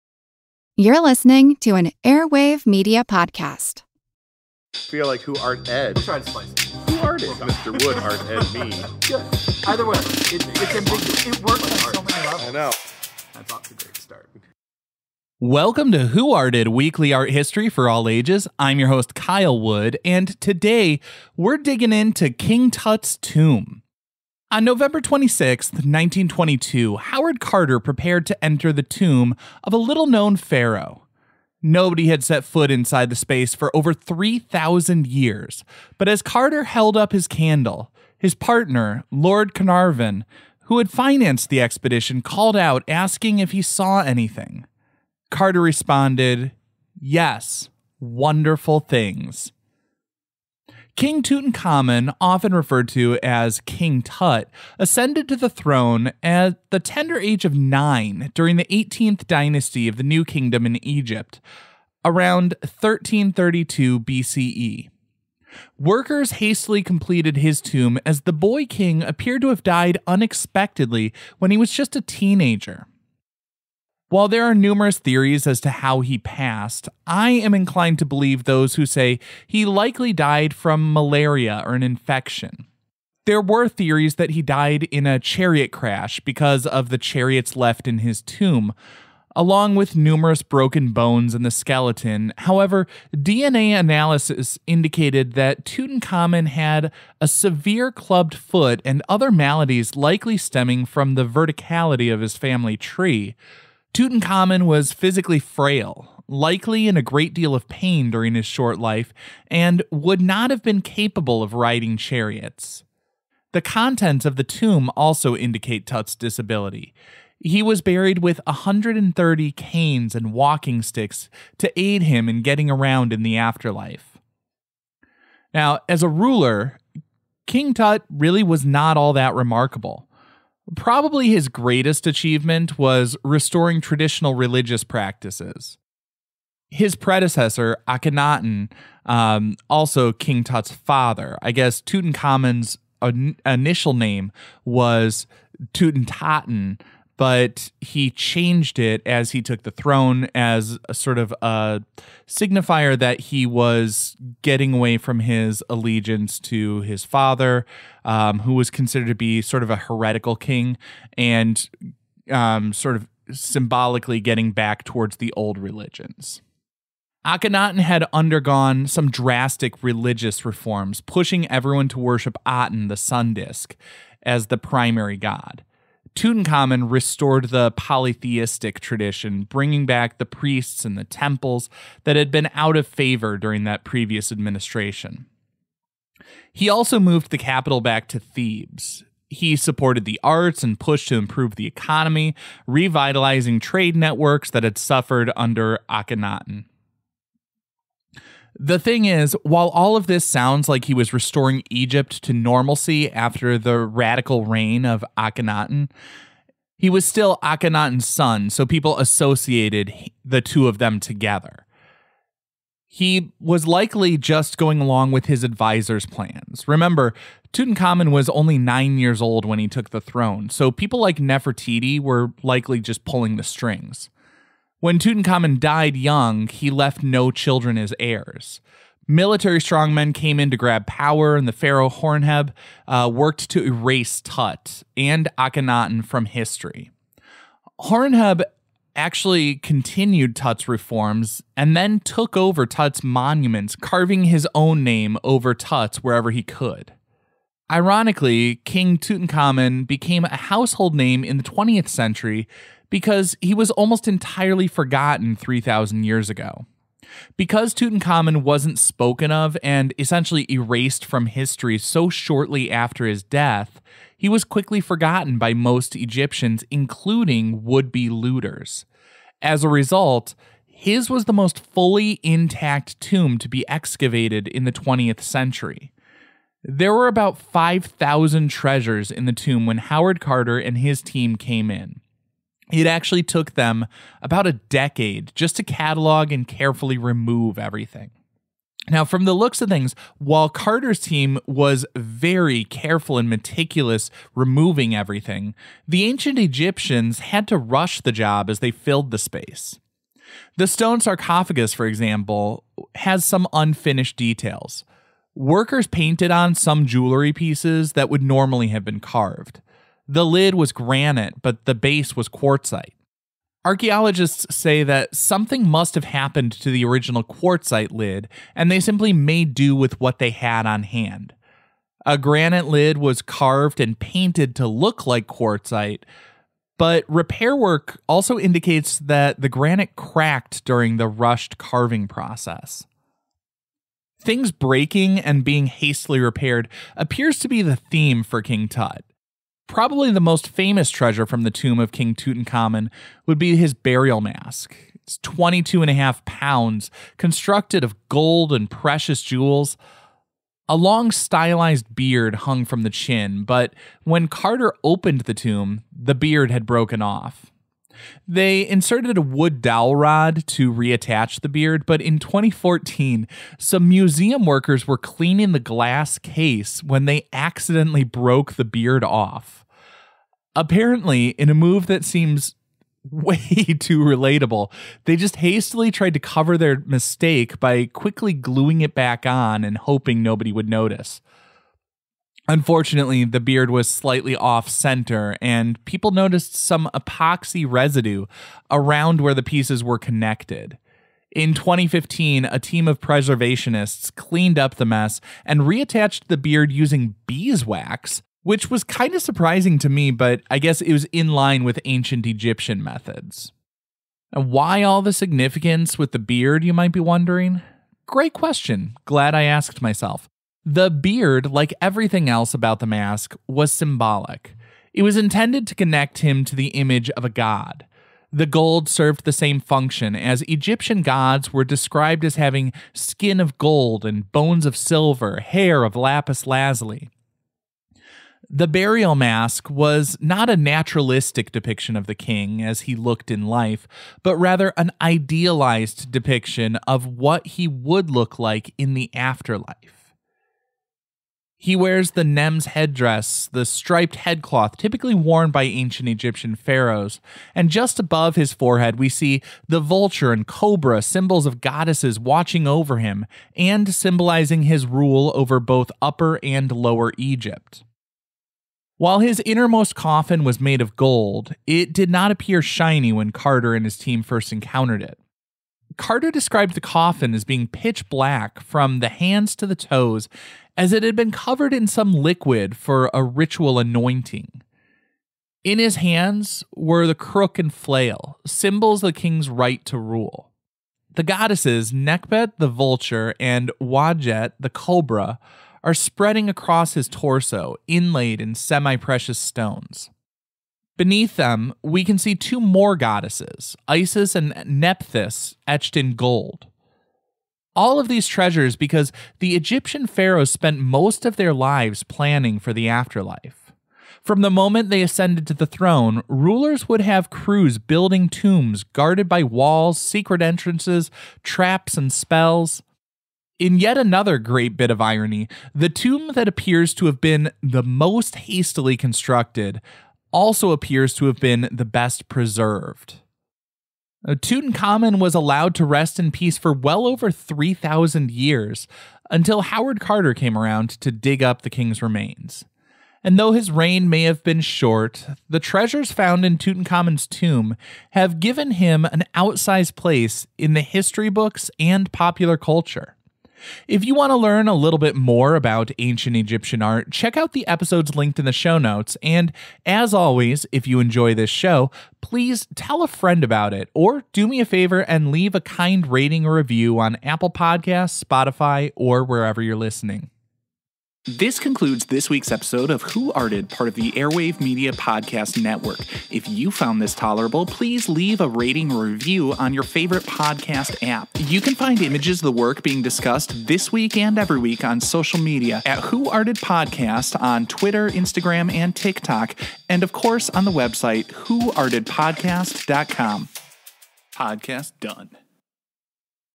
The Treasures of King Tutankhamun's Tomb
On November 26, 1922, Howard Carter prepared to enter the tomb of a little-known pharaoh. Nobody had set foot inside the space for over 3,000 years, but as Carter held up his candle, his partner, Lord Carnarvon who had financed the expedition called out asking if he saw anything. Carter responded, “Yes, wonderful things.” Though his reign may have been short, the treasures found in Tutankhamun’s tomb have given him an outsized place in the history books and popular culture.

| Rainbow Puppy Science Lab